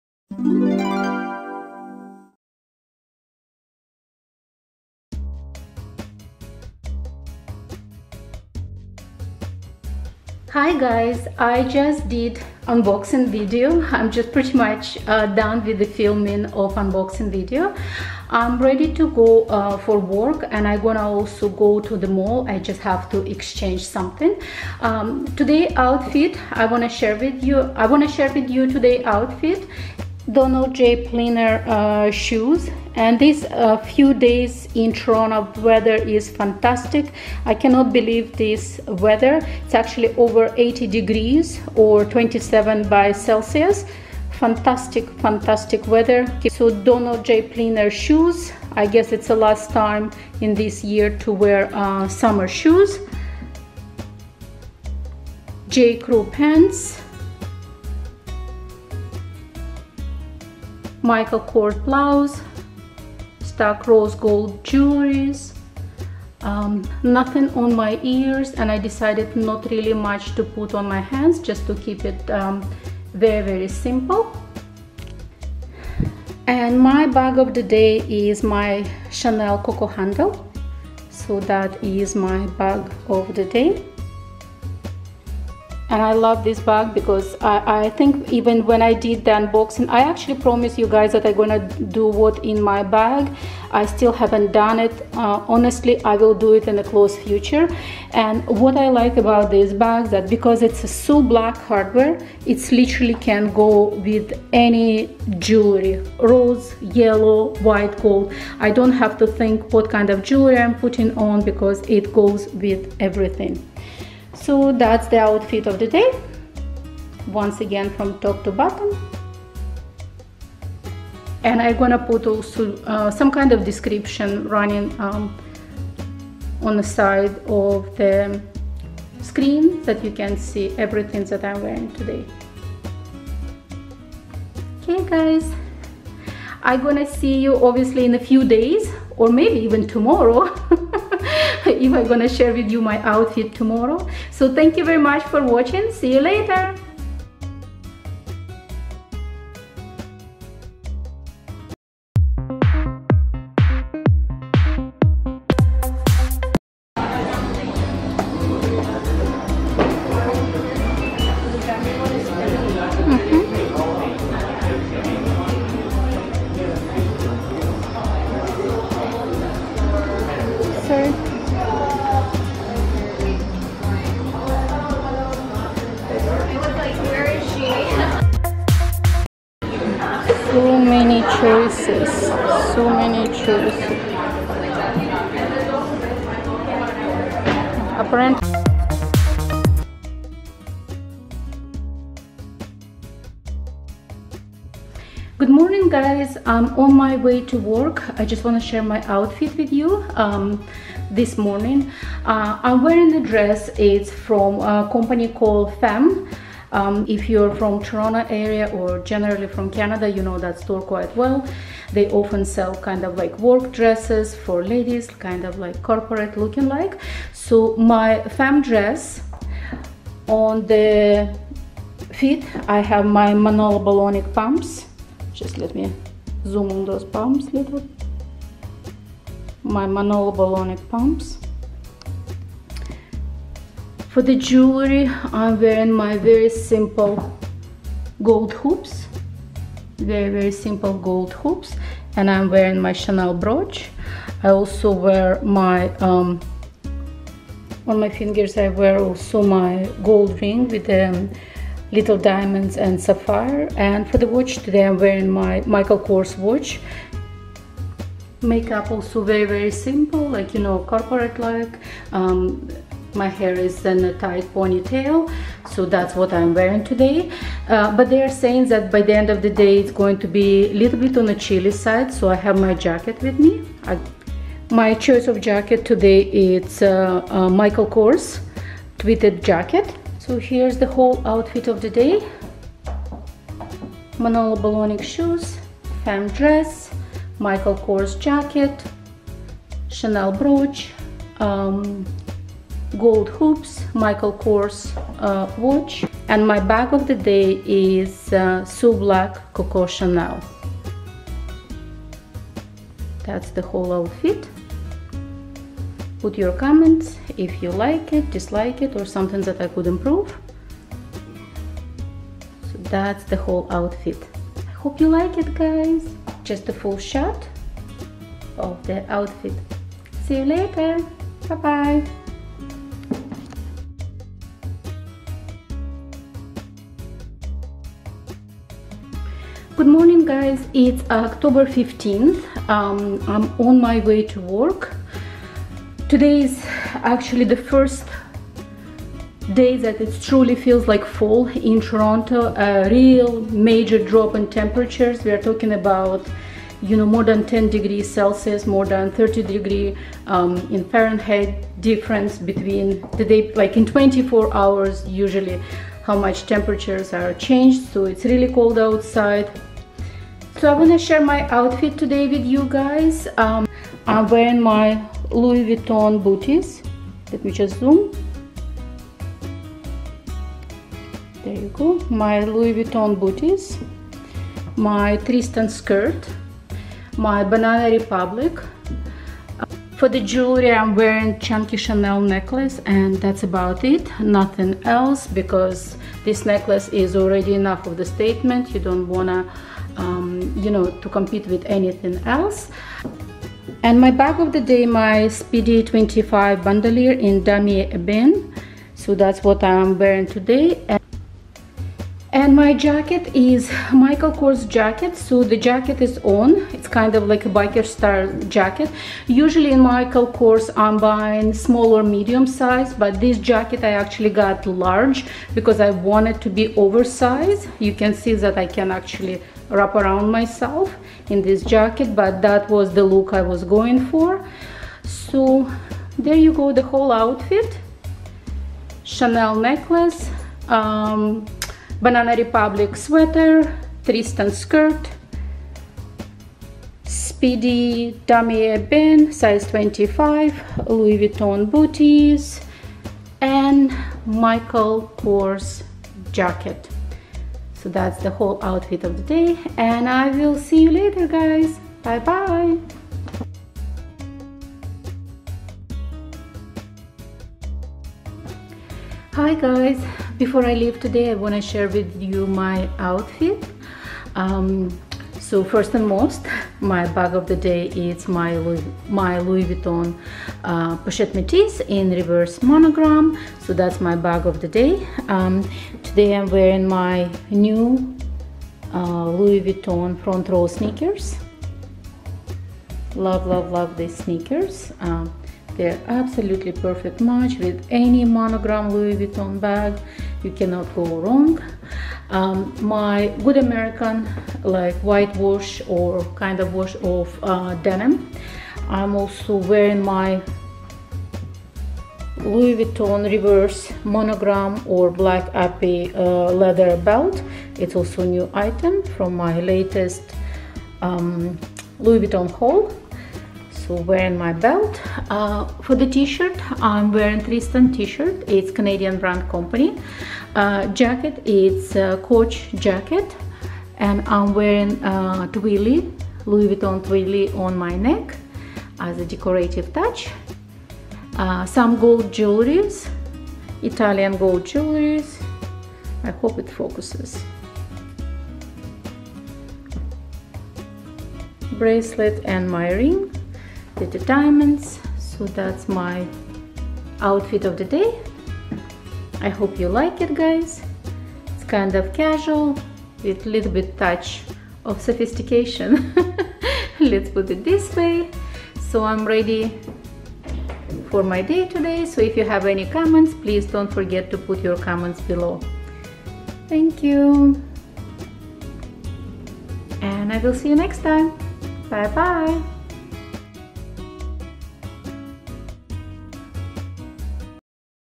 Outfits Featuring Chanel Coco Handle, LV Speedy, LV Pochette Metis, LV Frontrow sneakers | OxanaLV
Hello Ladies!

I love designer hand bags. Every single one is an art, in my point of view. Let me share my thoughts with you.

Cheers,
Oxana LV